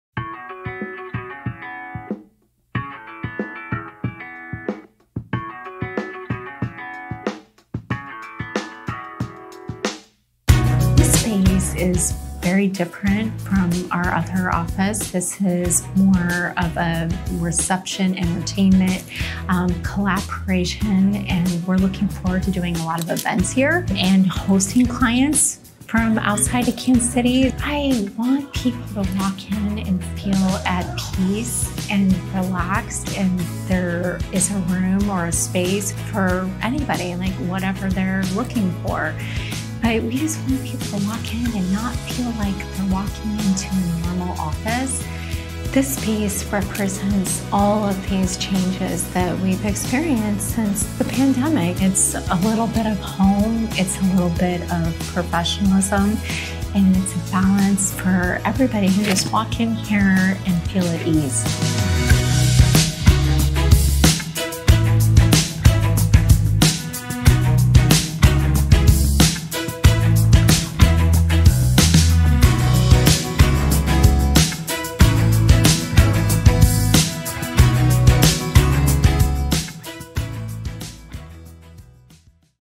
Step Inside Our New Office | Modern Workspace | Digital Marketing Agency 20-Year Anniversary
Join JSMM in celebrating our 20th-year anniversary with a grand reveal of our brand-new office, meticulously crafted to foster connection and collaboration.

In this exclusive tour, you'll get a firsthand look at the transformation that represents the changes we've experienced since the pandemic. Our CEO, Valerie Jennings, describes it as a perfect blend of homey comfort and professional ambiance, creating a welcoming atmosphere for all. JSMM can't wait to host events, welcome clients, and enjoy the relaxed, peaceful vibe of our modern workspace.

Watch the video now for an exclusive tour of our stunning new space. 🏢🔑